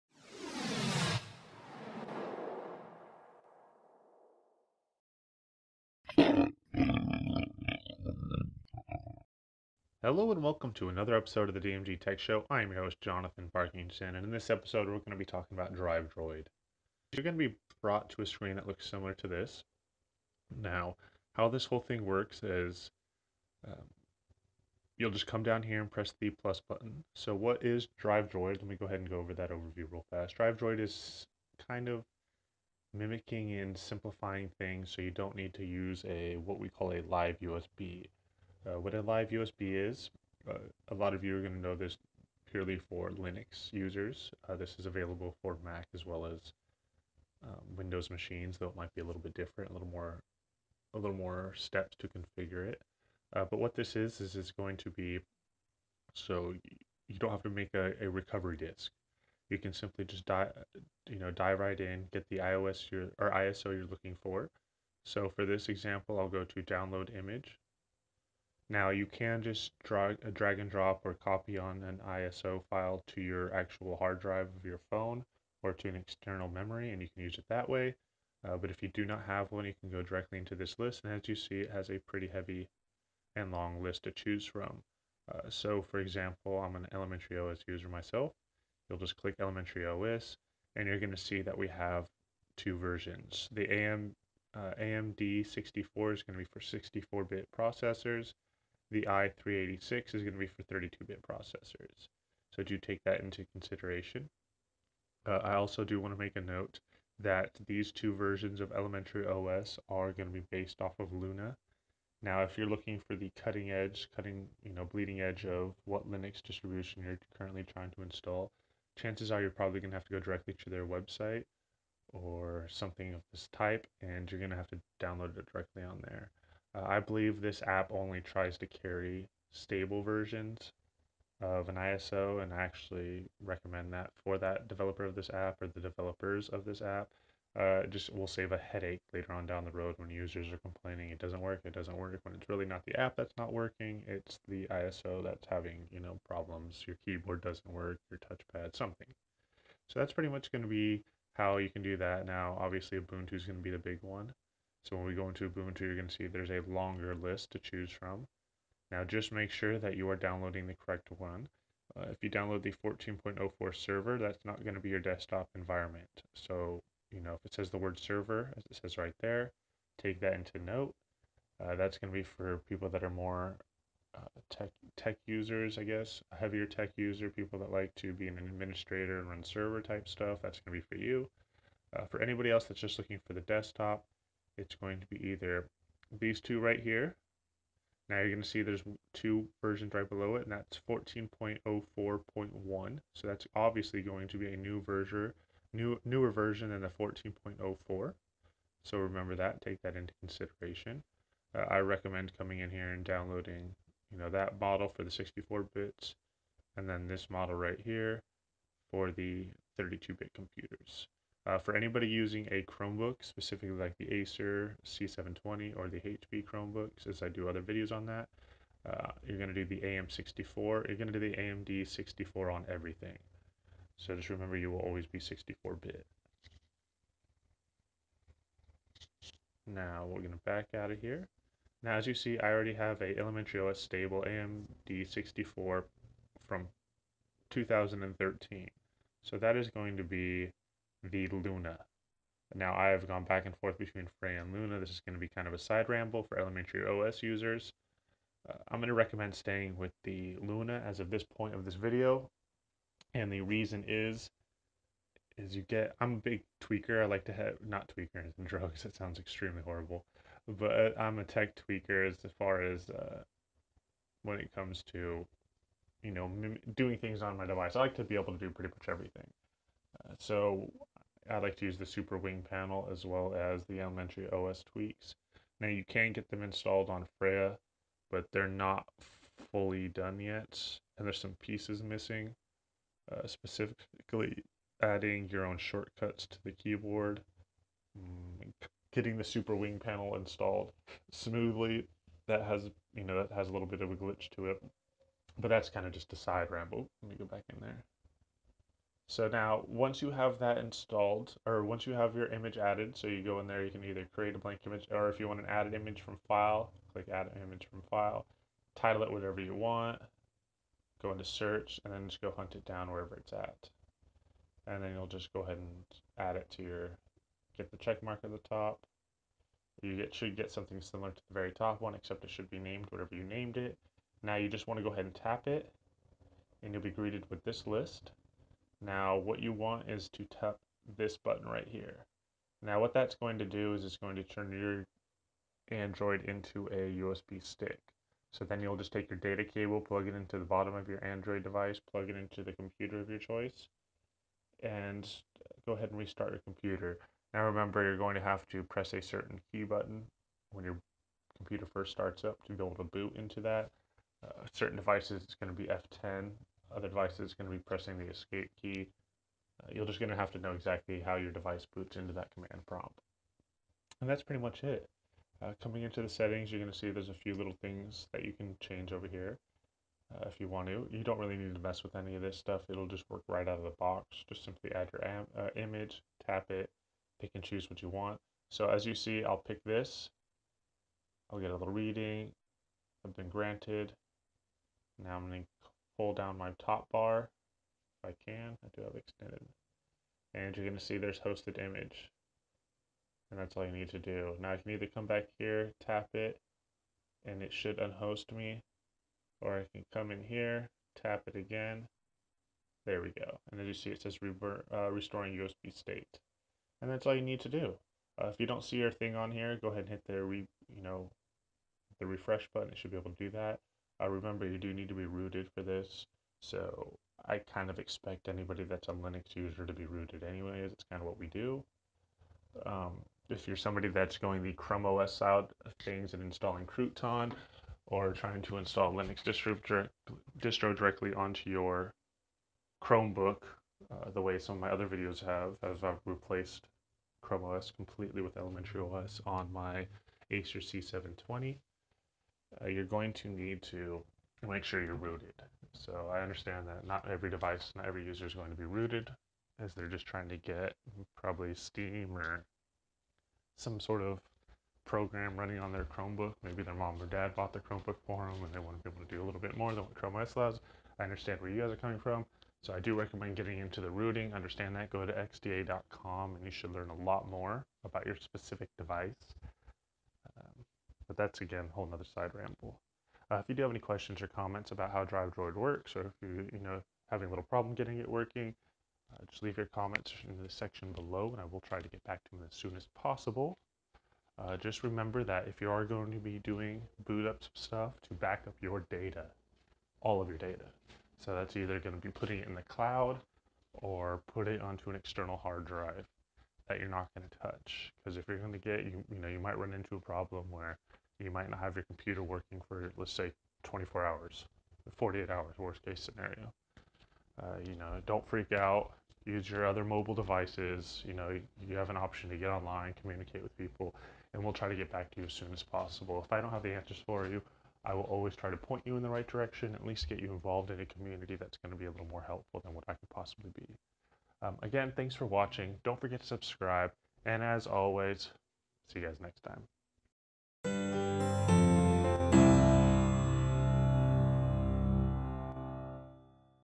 Android 101: Drivedroid LiveUSB App **Root Required**
Drivedroid is the ultimate application for Android users who are also fans  of Linux. With this app you can install any ISO file directly from your Andoid device straight onto a bootable PC.